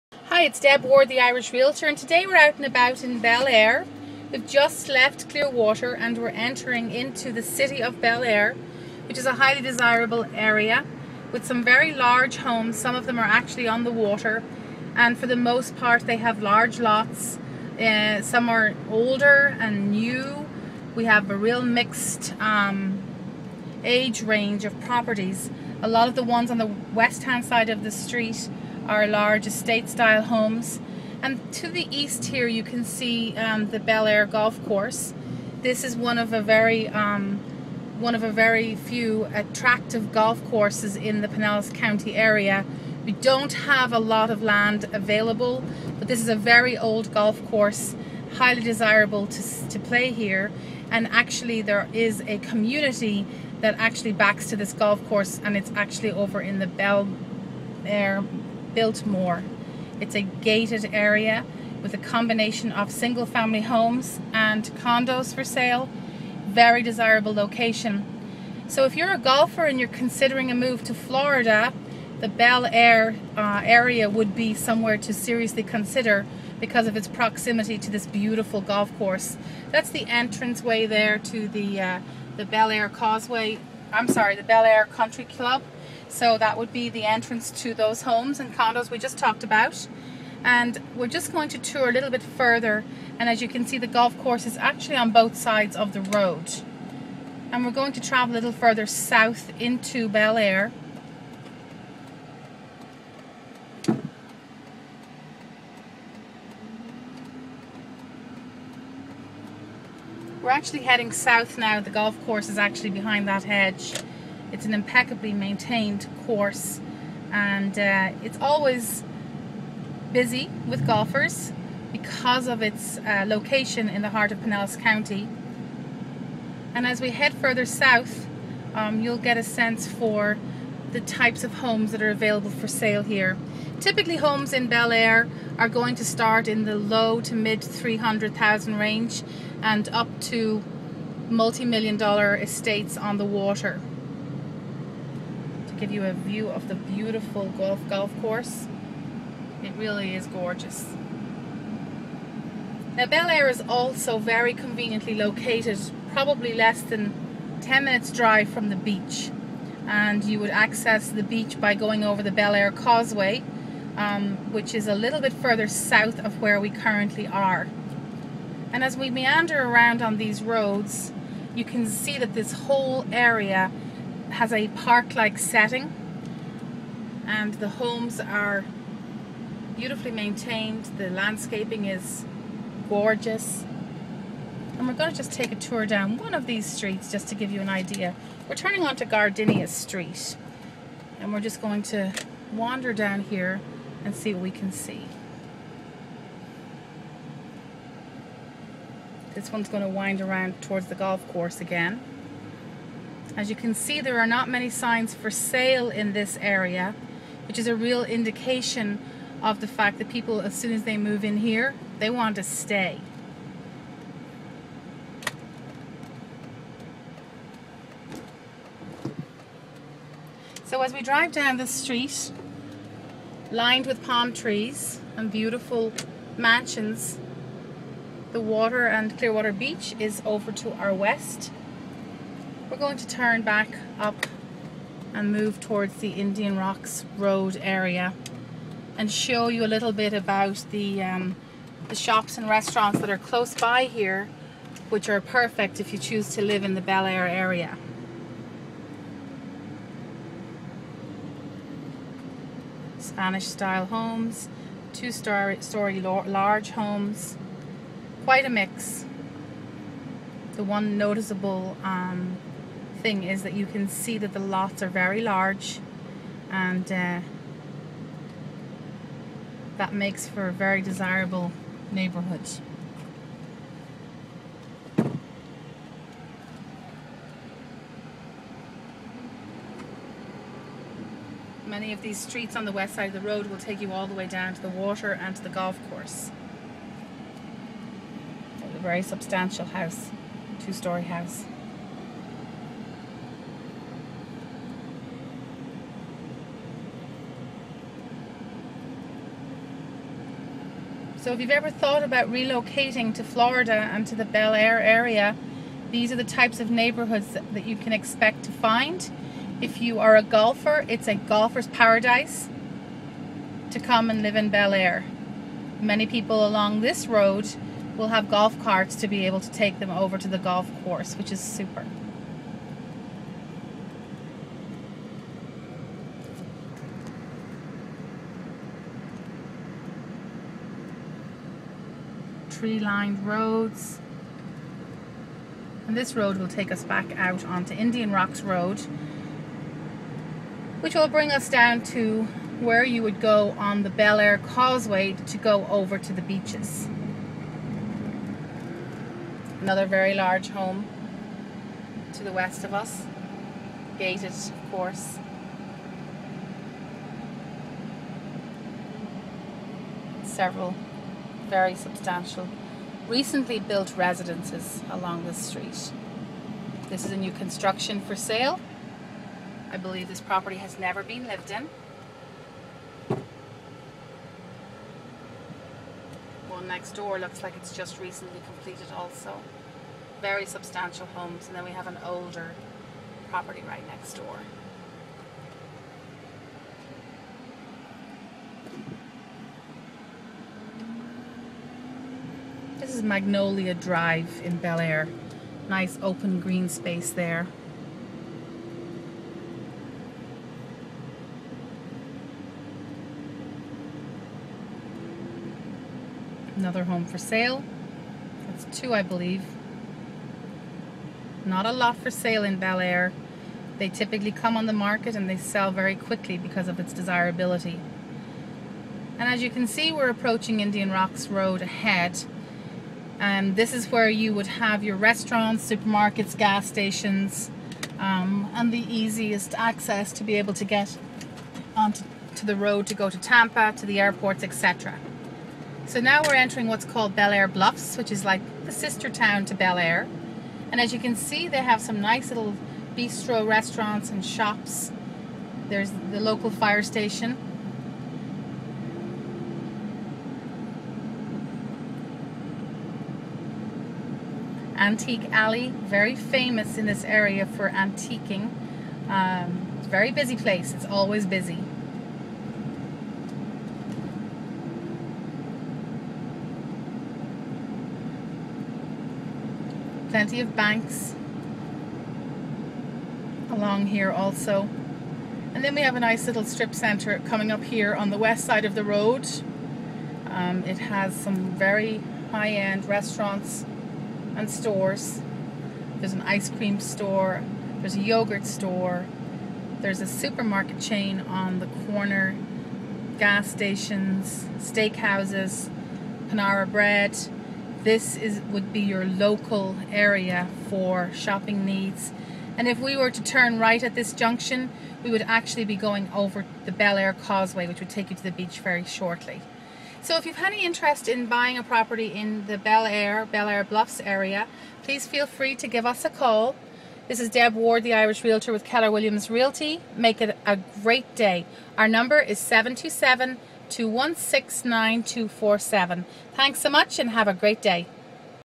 Belleair, Florida Real Estate
- come along with Deborah Ward, Keller Williams Realty as she drives through prestigious Belleair, Florida.  Belleiar is located just south of Clearwater, and is just a short drive from the great Gulf beaches of Clearwater Beach, Belleair Beach, and Indian Rocks Beach., If you have any questions about real estate in the Belleair, Clearwater, or Gulf Beaches area, please.